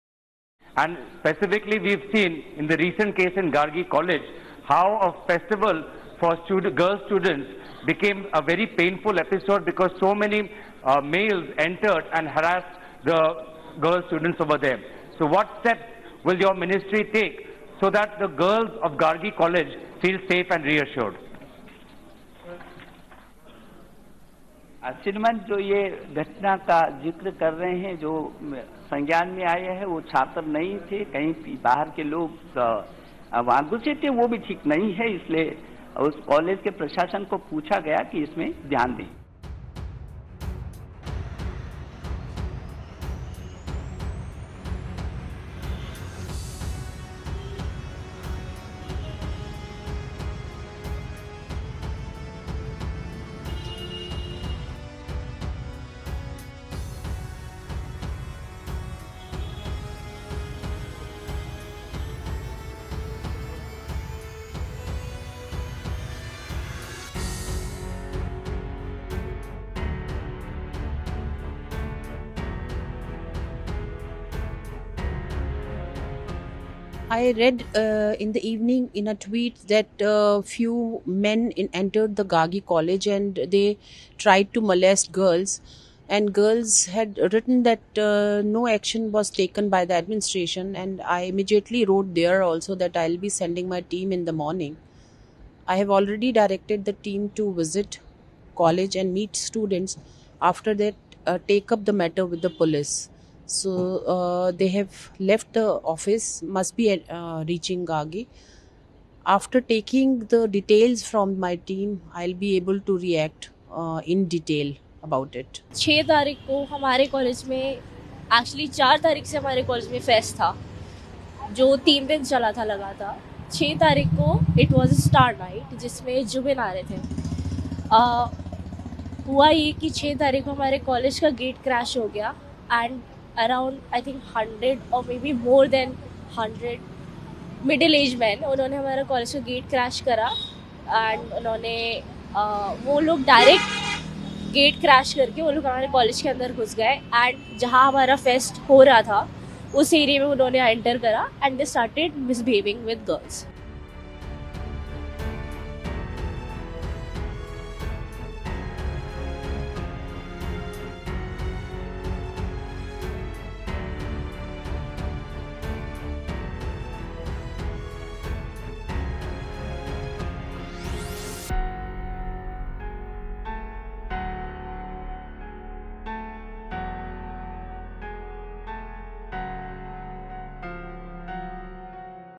Watch l Gargi college molestation raised in Lok Sabha; NCW team visits campus
The Gargi college molestation issue was raised in the Lok Sabha by Congress MP Gaurav Gogoi. Union HRD Minister Ramesh Pokhriyal said that those involved in the incident were outsiders and added that the college administration has been asked to act tough. Meanwhile, a team of the NCW also visited the college and spoke to students.